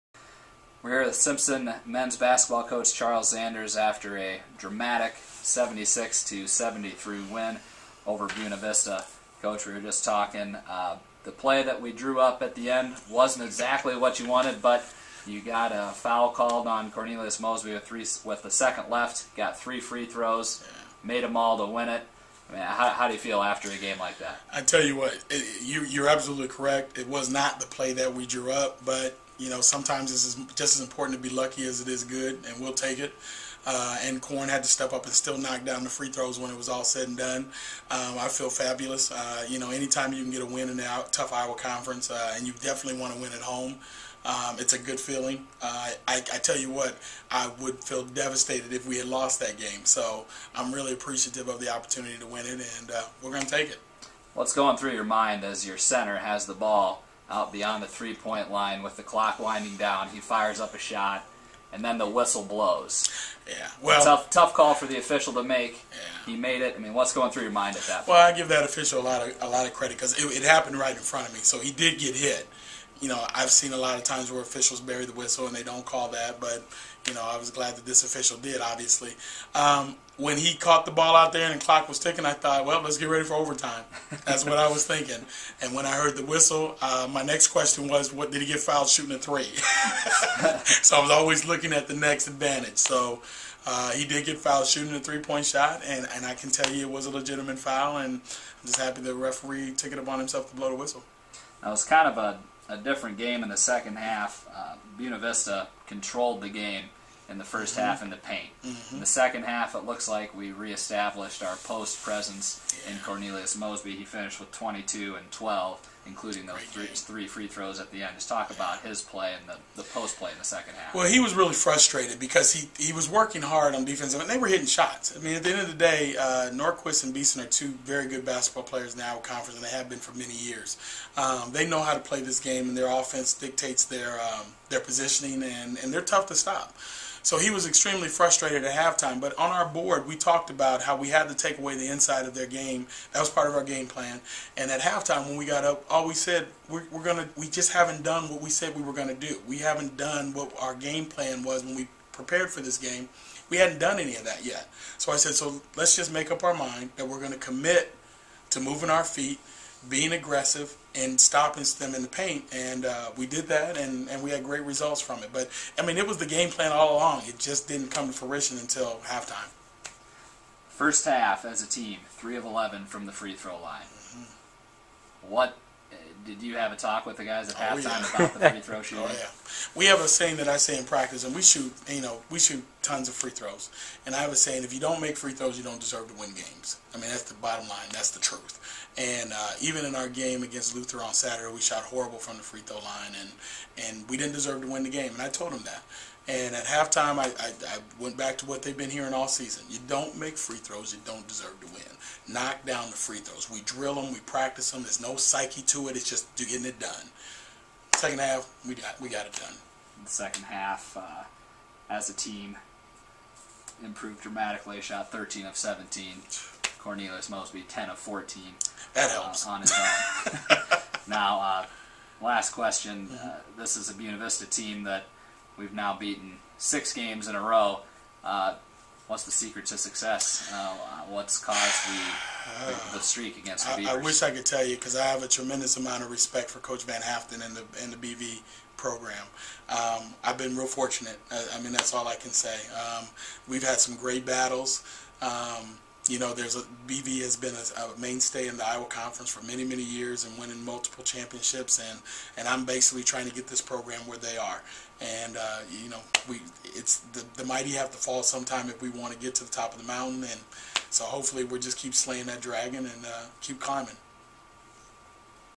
Charles Zanders Postgame Interview Buena Vista
Head men's basketball coach Charles Zanders talks about Simpson's dramatic 76-73 win over Buena Vista on Wednesday, Feb. 8, 2012.